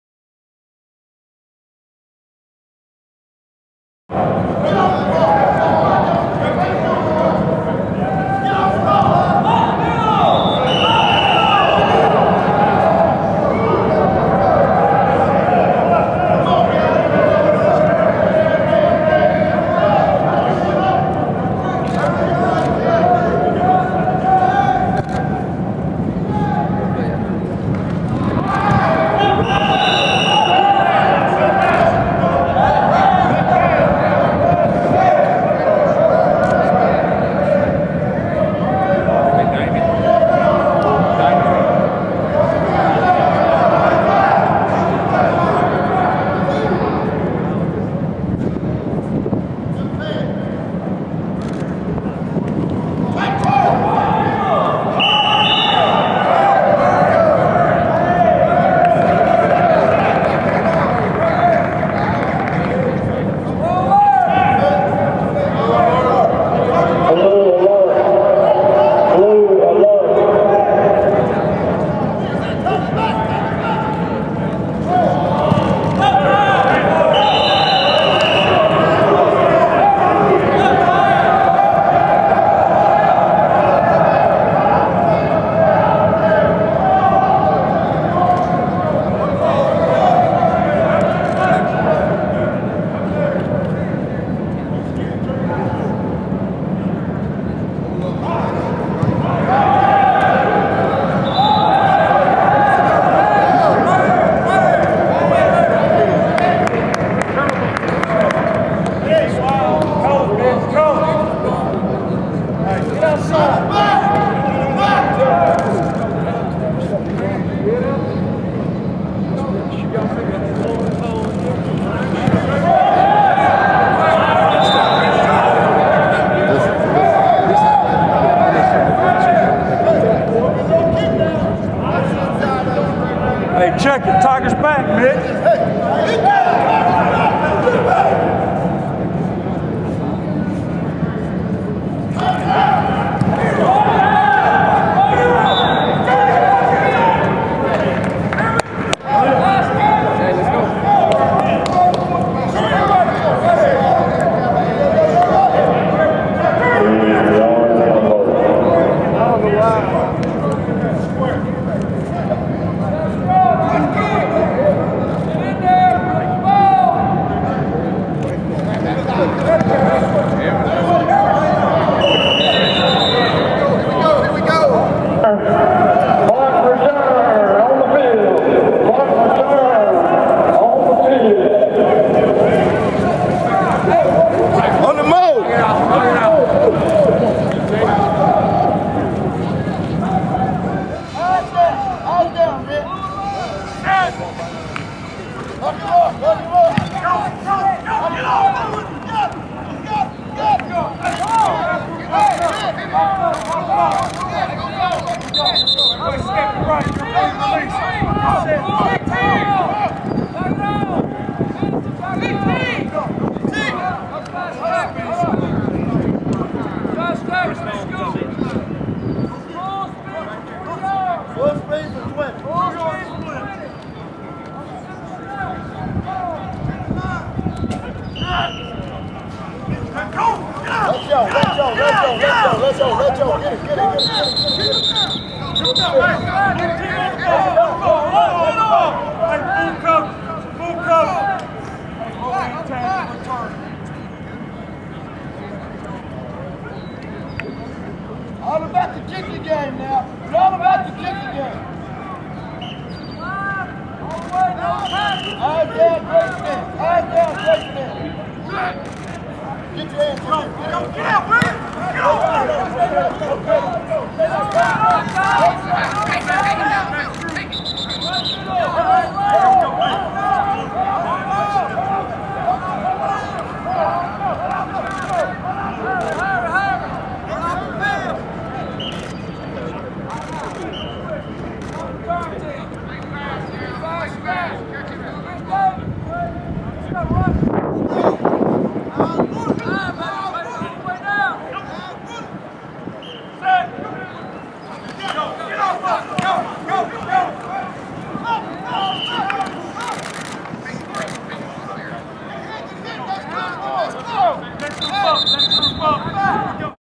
Auburn practice: Special teams 4-3-13
Video highlights from Auburn's spring practice on Wednesday, April 3, 2013 including field goal kicking and punt return by AuburnUndercover.com.